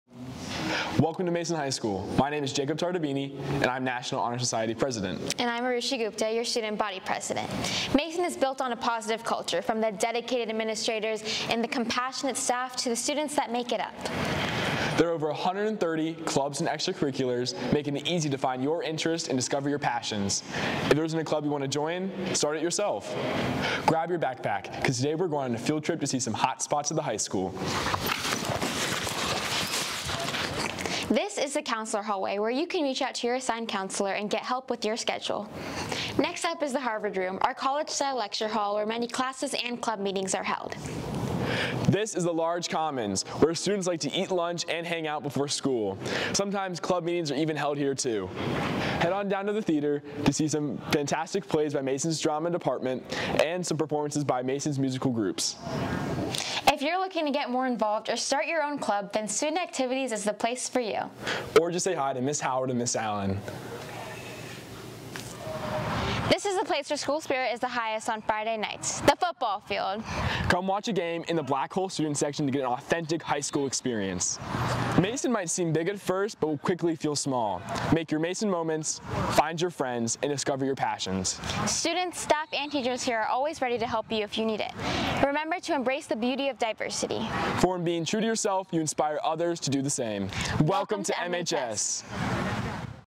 Welcome to Mason High School | 2023-24
Welcome to Mason High School! Watch this video (which is written, produced, and directed ALL by MHS students) to learn more about our school and see some of the most popular places!

Special thanks to Eli Clayson, Arushi Gupta, Jacob Tartabini, Ms Allen, and Mr Mckay!